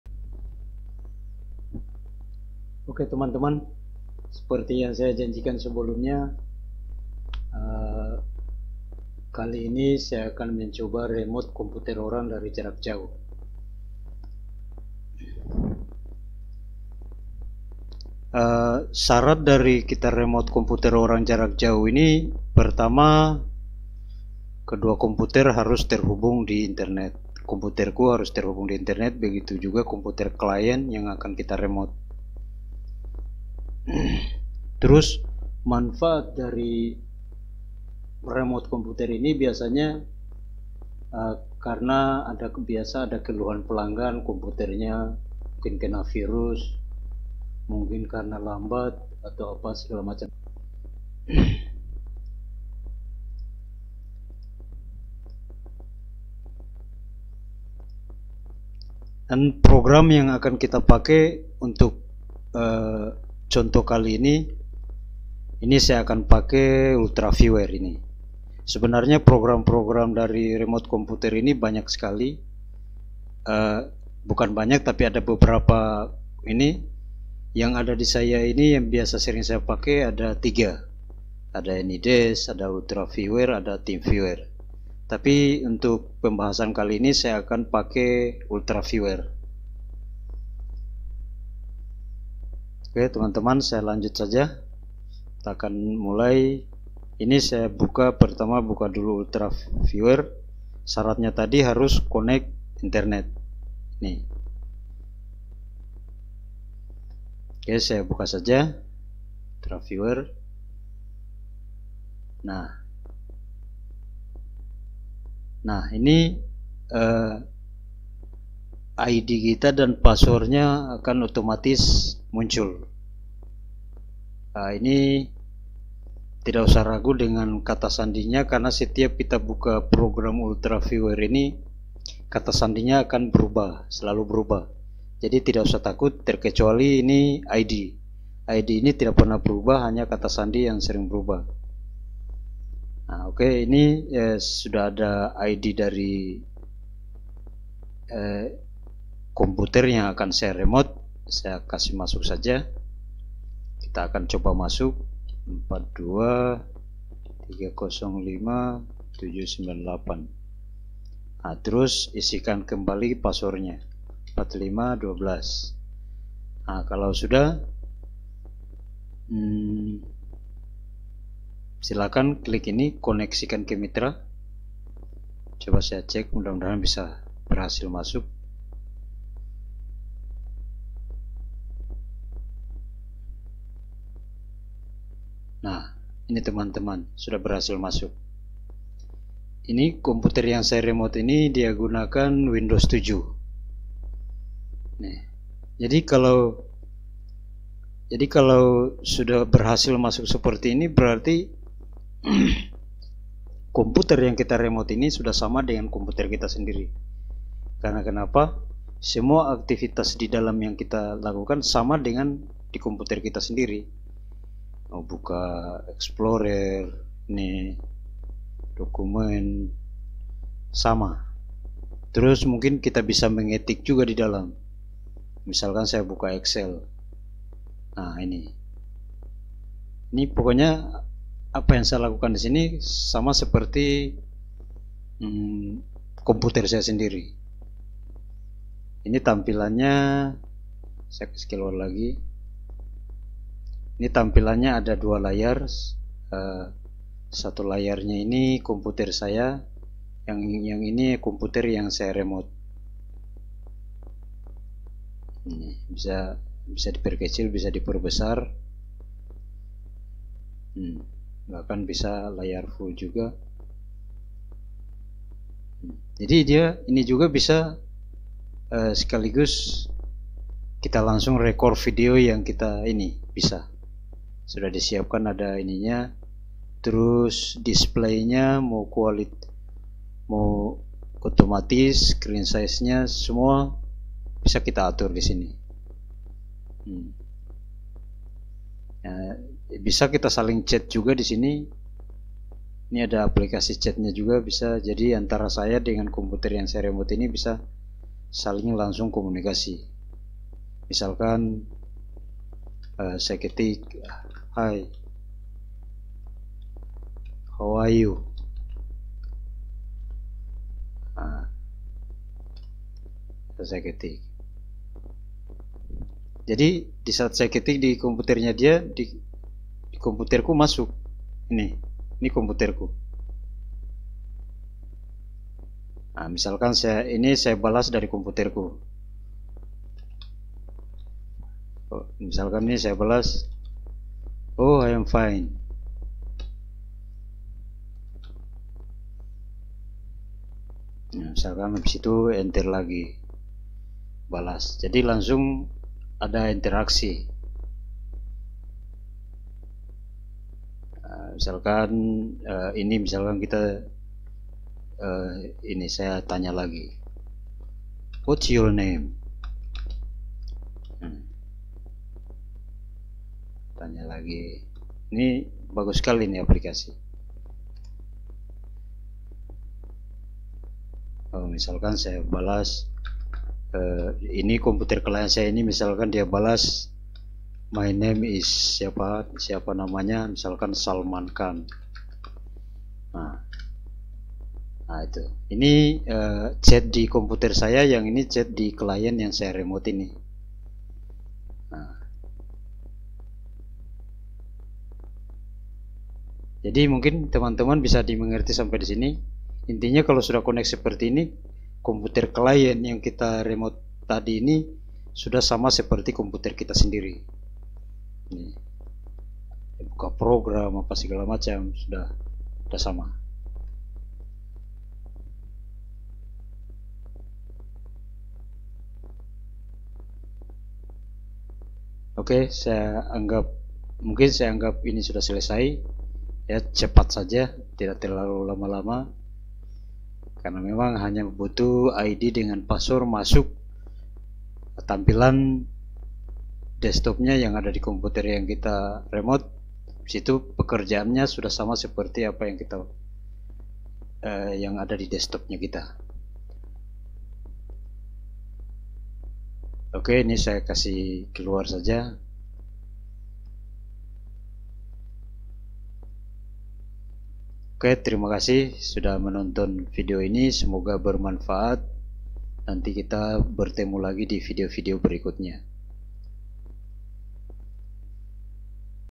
Cara remote komputer dari jarak jauh
cara remote komputer dari jauh sekarang ini sudah tidak susah lagi. Yang penting antara komputer kita dengan komputer client sama-sama terhubung via internet. Di Video ini saya sudah jelaskan caranya menggunakan aplikasi ultra viewer. Penjelasannya ringan dan pasti cepat di mengerti. Semoga bermanfaat